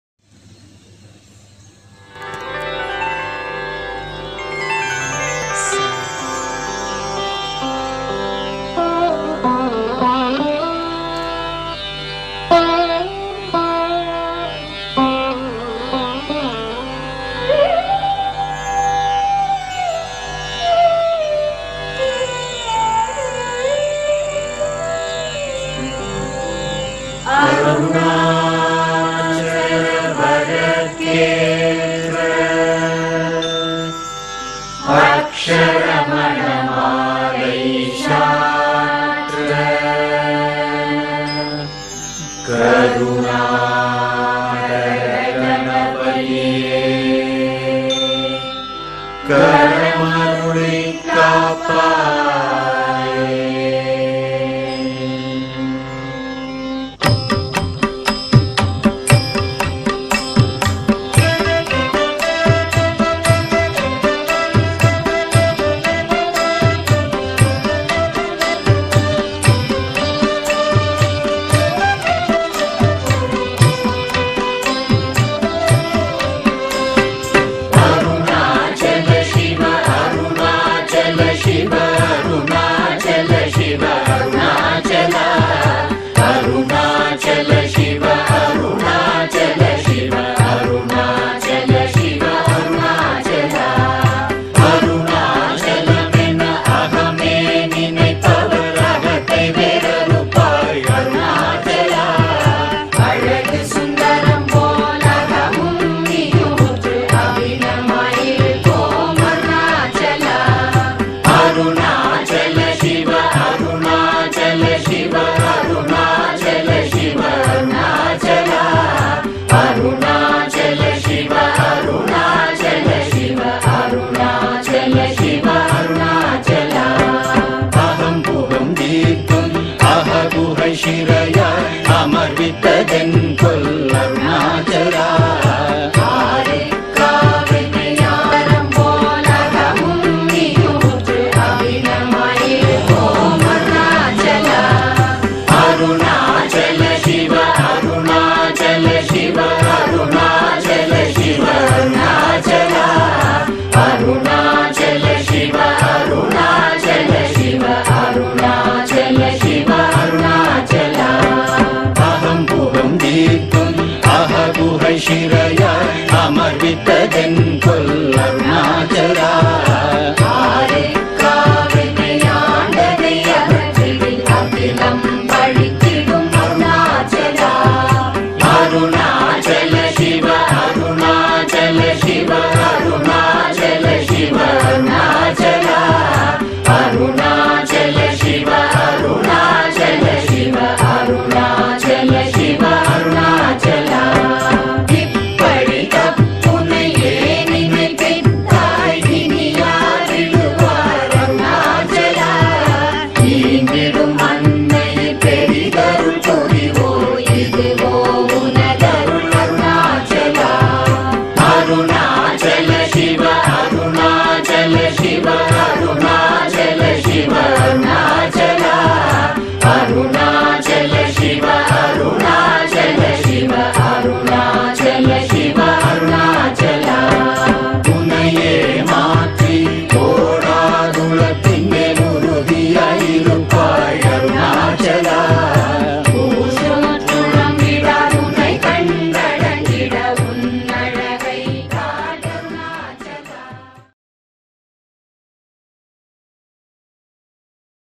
Arunachala thiruvanamalai trip
Lord Arunachala , Ramana Ashram, Peacock, Girivalum Thiruvanamalai  swamigal seshadri ashram.  lingams video